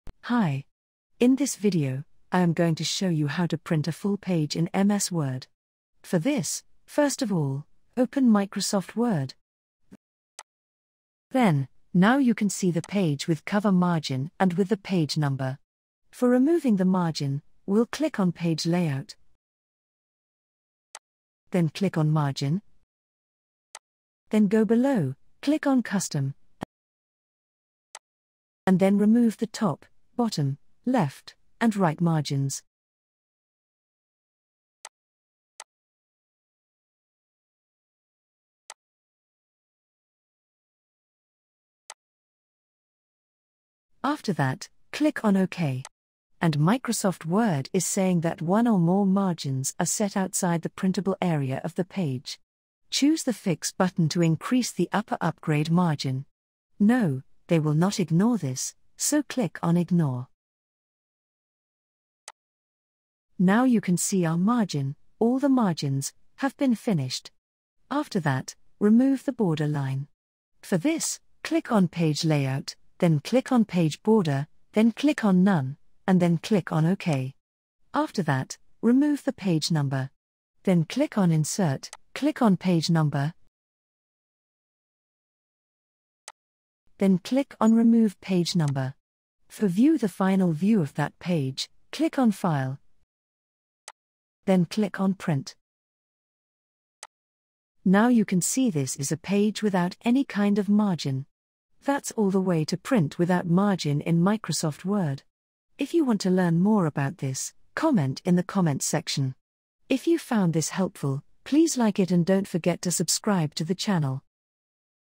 How To Print Full Page In Microsoft Word | Quick & Easy
Master the art of printing full pages in Microsoft Word with our expert tutorial! Learn essential tips and techniques to ensure your documents are perfectly printed from edge to edge. Say goodbye to incomplete prints and hello to professional-quality documents. Start printing with confidence today!